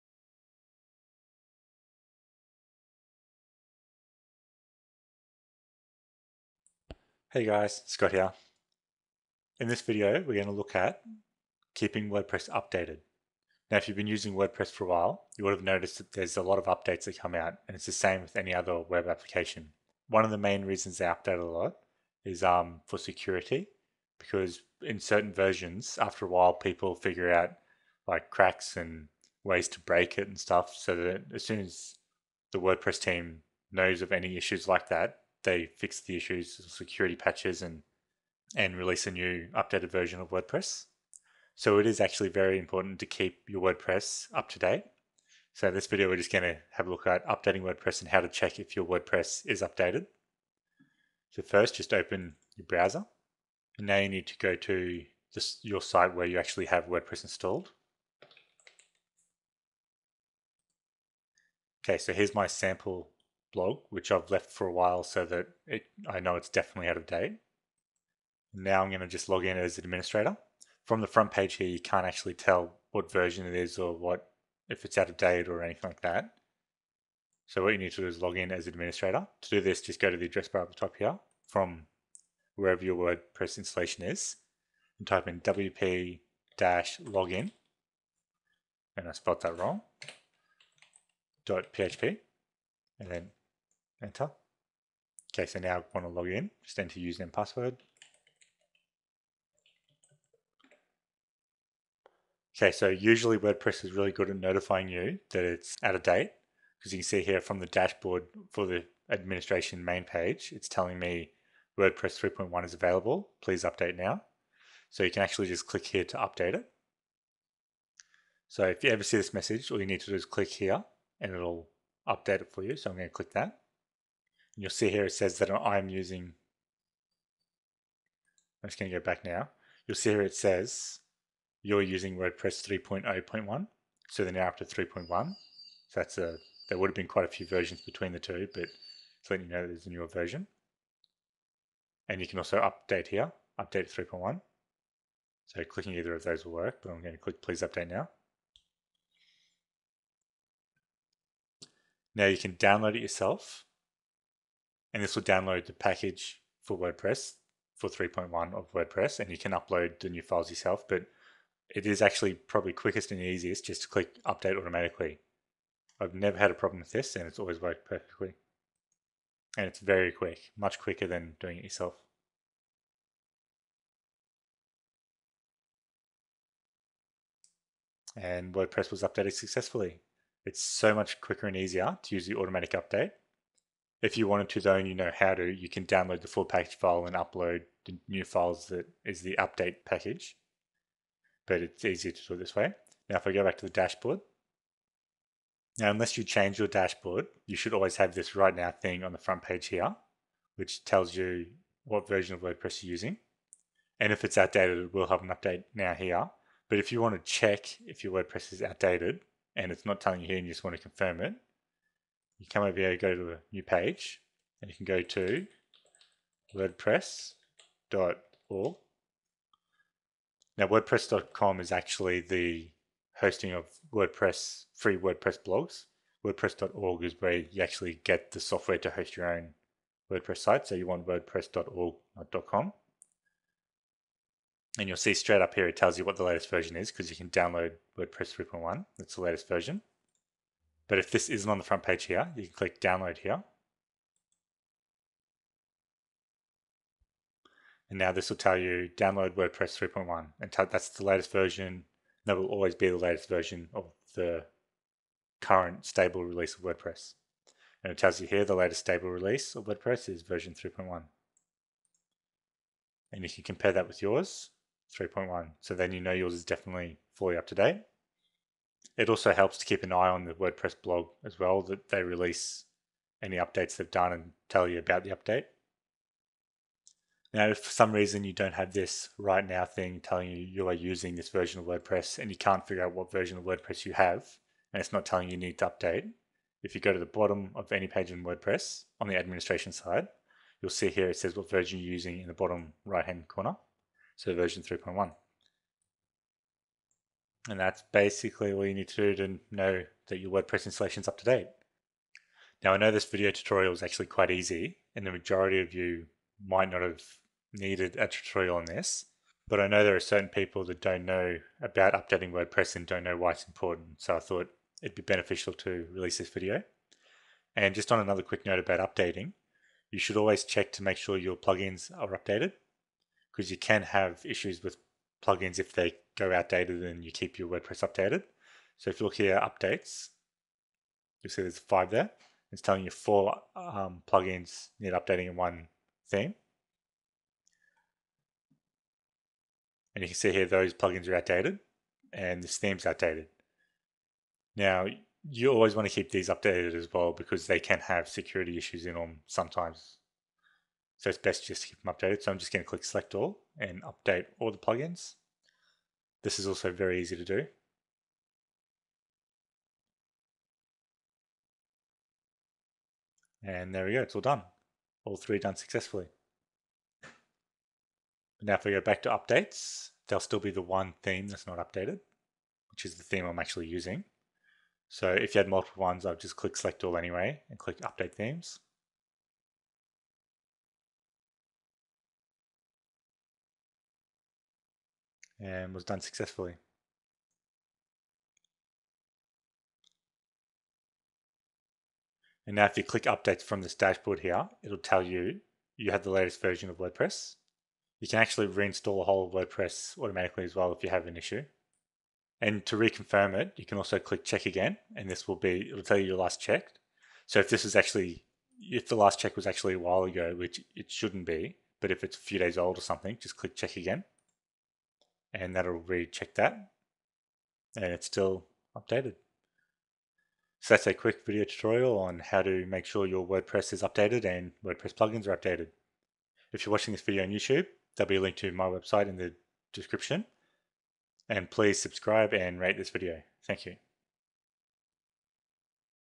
How To Update WordPress And Why
Above is a link to my website where the tutorial for this video is located.

Updating WordPress is very important and you should always keep on top of it. Not only does it prevent any type of unwanted security issues but WordPress updates can also include new features, improved performance changes and any other helpful changes. So make sure to keep WordPress updated.